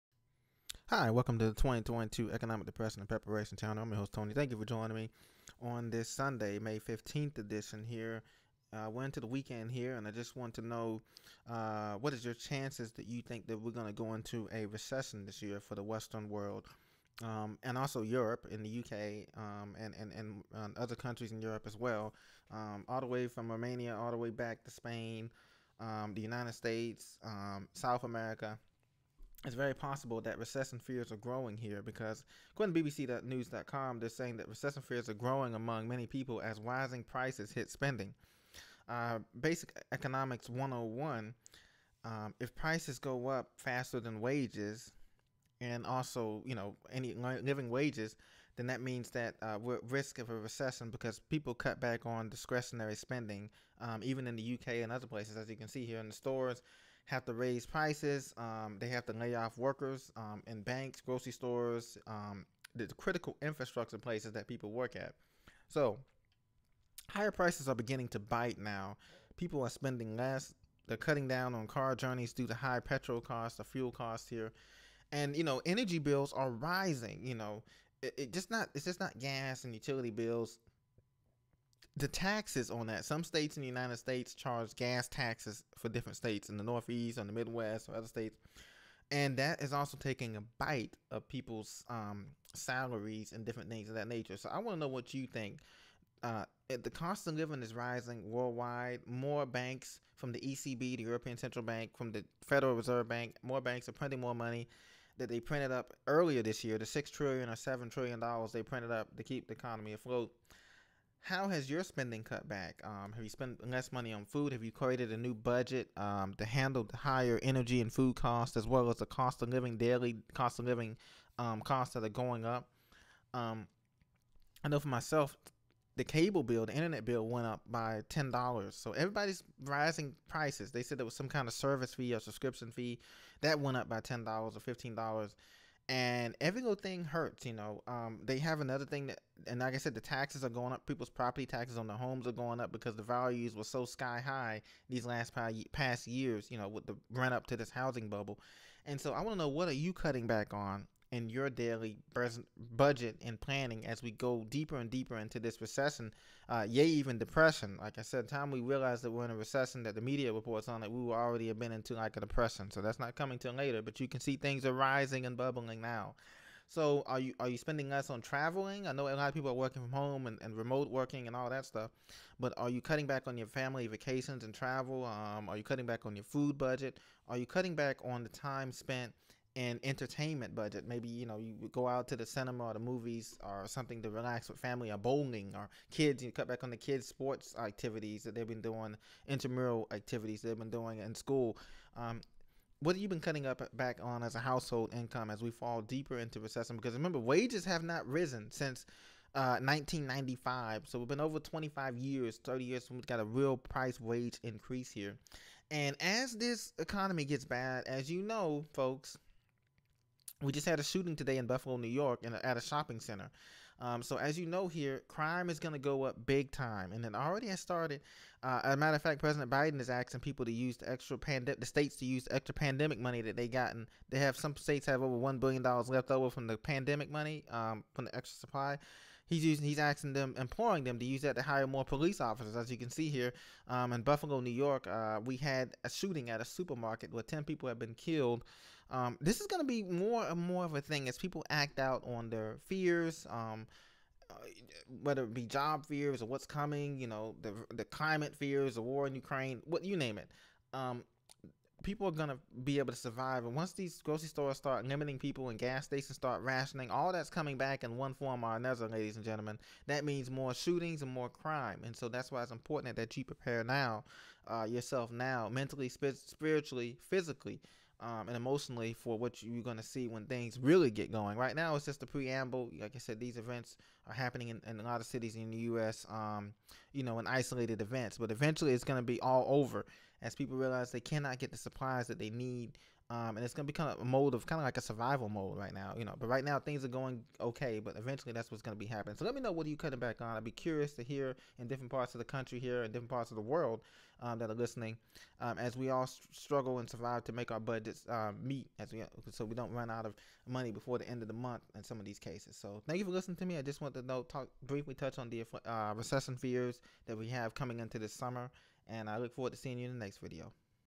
50/50 Chance of Recession or a Depression?
Are we headed to a minor recession or a major depression? You be the judge!